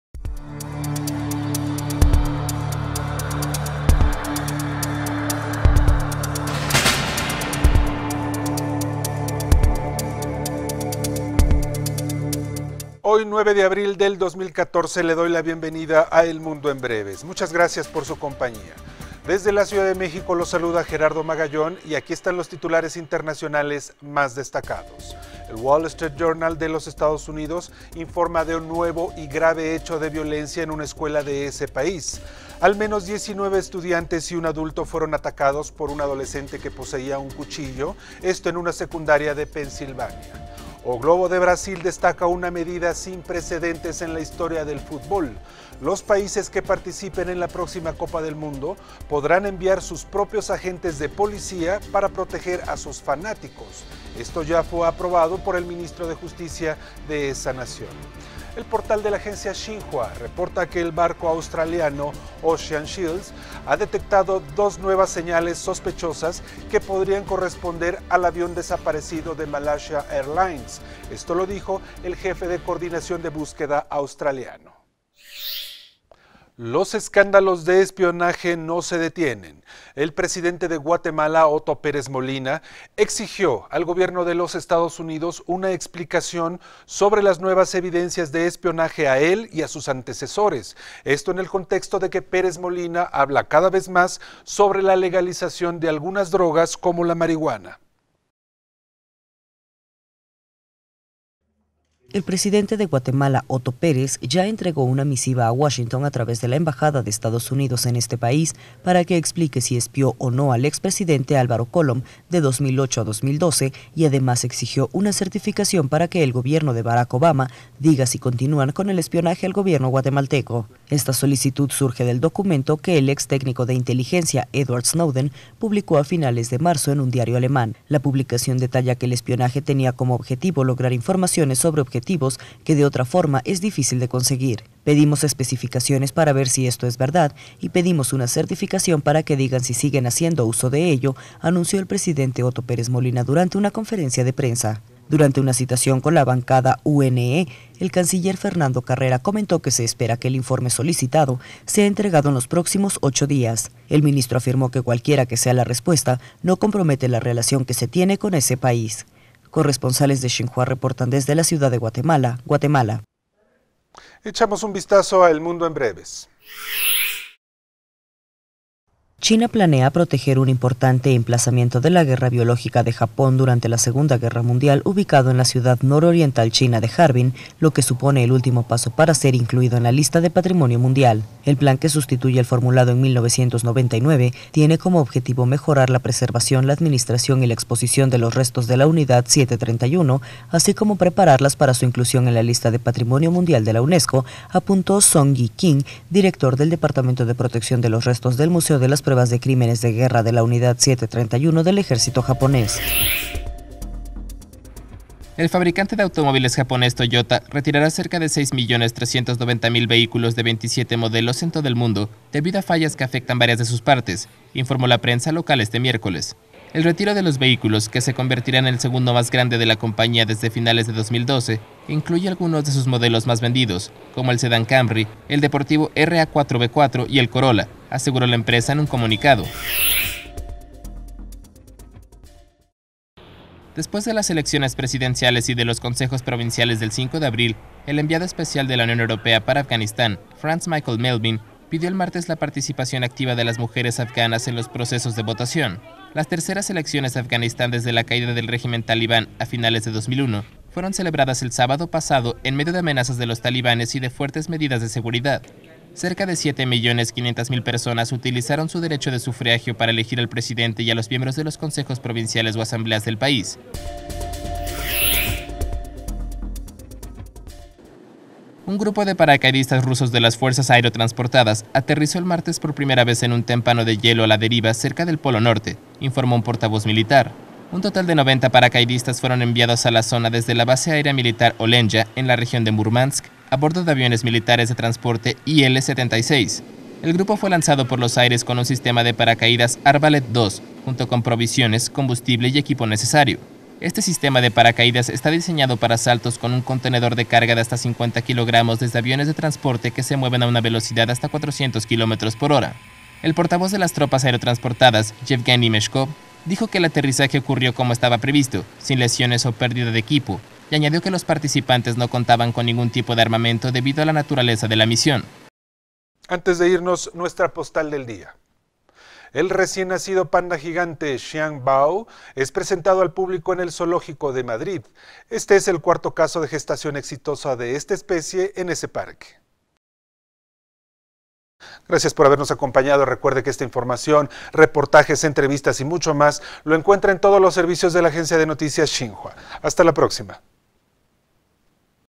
El Mundo en Breves 04092014
EL MUNDO EN BREVES
Intro: Resumen informativo con enfoque mundial
Fecha: 09/04/2013
Locación: Ciudad de México, México
Duración: 5:38
Idioma: Español (sonido natural)
Fuente: XinhuaTitulares
Titulares
El Wall Street Journal de los Estados Unidos da cuenta de un nuevo indecente de violencia en las escuelas de ese país, al menos diecinueve estudiantes y un adulto fueron heridos cuando una adolescente los atacó con cuchillos en al secundaria regional de Franklin Pensilvania.
O Globo de Brasil informa que los países que participen en la copa del mundo que se efectuará en ese país, podrán enviar agentes de policía para que protejan a sus fanáticos, esta medida sin precedente ha sido aprobada por el ministerio de justicia de ese país.
El portal de la agencia Xinhua informa que El barco australiano Ocean Shield ha detectado dos nuevas señales sospechosas en una búsqueda intensificada del avión desaparecido de Malaysia Airlines, dijo el jefe de coordinación australiano-
Principal
7. Ciudad de Guatemala, Guatemala
Presidente de Guatemala pide explicación de espionaje a EE.UU.
Breves
4. Harbin, China
China planea incluir ruinas de Unidad 731 como patrimonio mundial
11. Tokio, Japón
Toyota retirará más de 6 millones de automóviles en todo el mundo
12. Kabul, Afganistán
Enviado de la UE a Afganistán pide a mujeres participar activamente en elecciones
13. Moscú, Rusia
Paracaidistas rusos aterrizan en el Polo Norte por primera vez